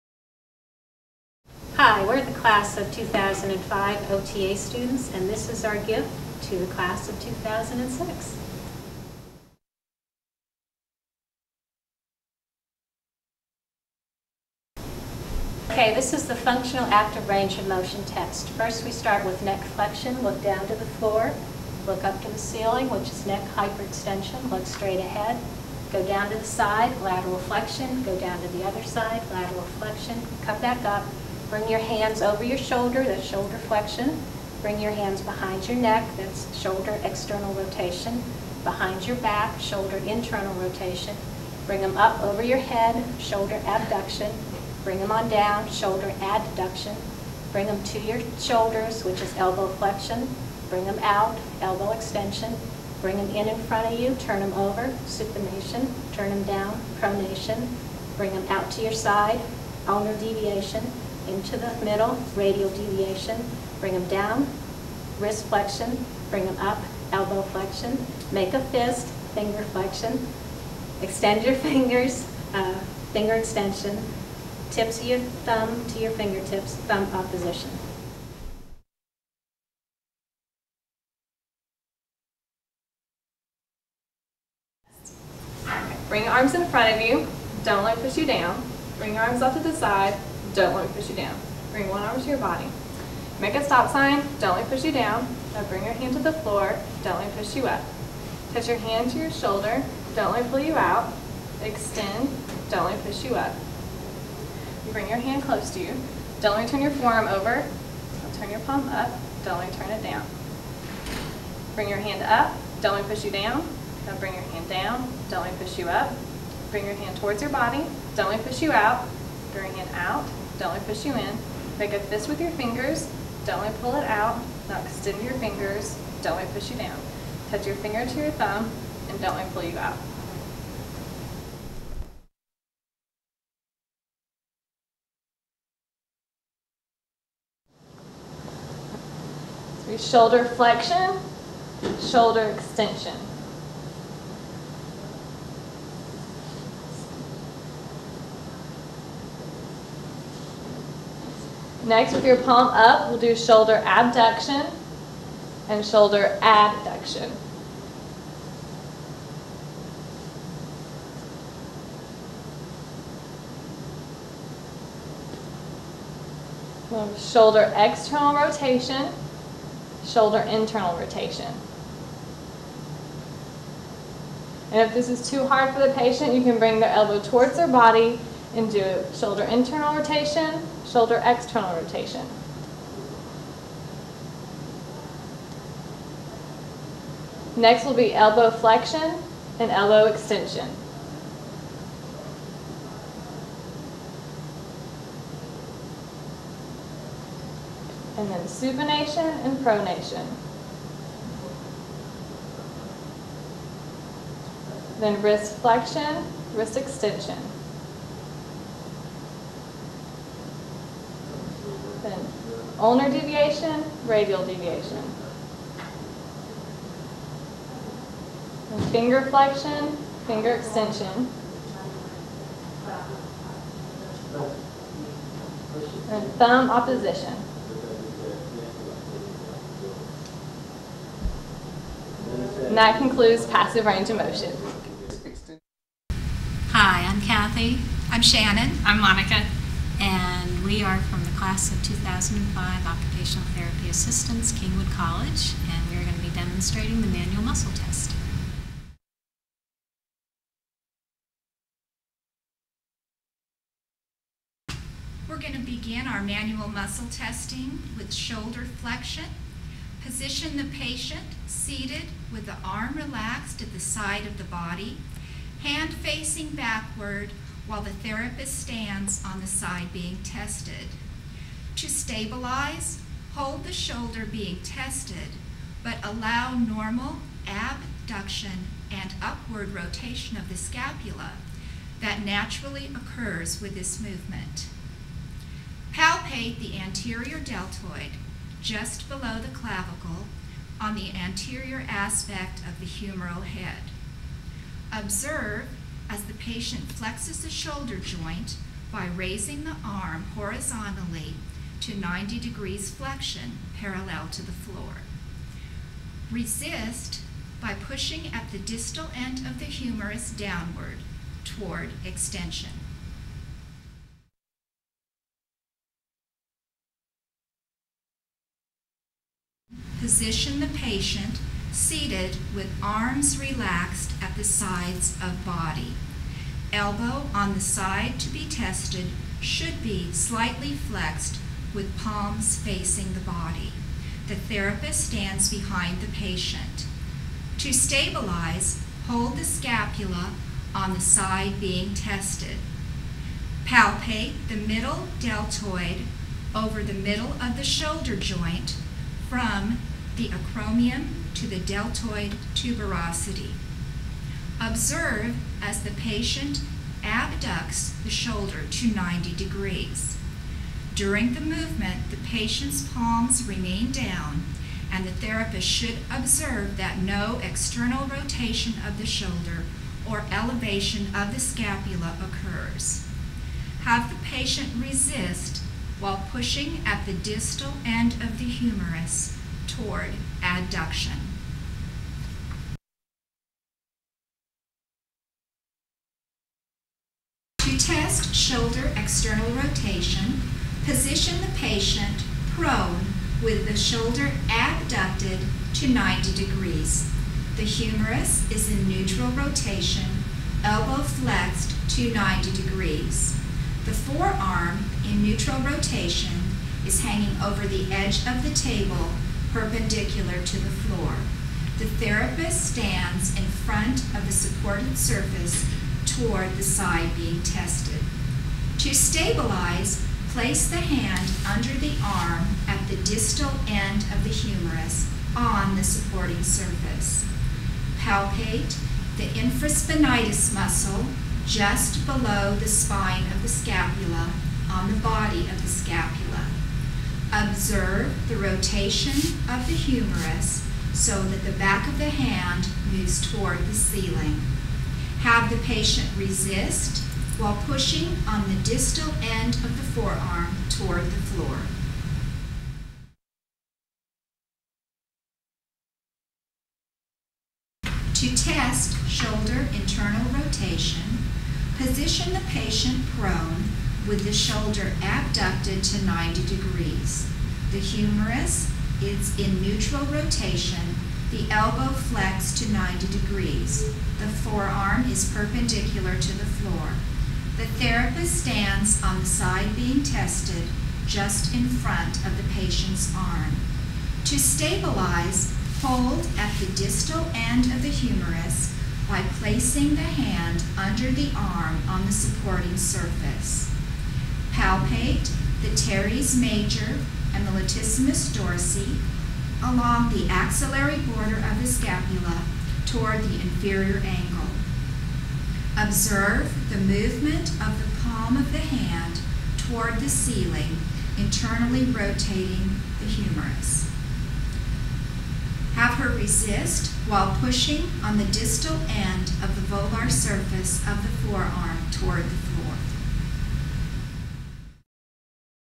OTA Class of 2005 Video, Part 1
Functional Active Range of Motion, Functional Manual Muscle, and Passive Range of Motion Tests conducted by students of the Occupational Therapy Program at Lone Star College-Kingwood.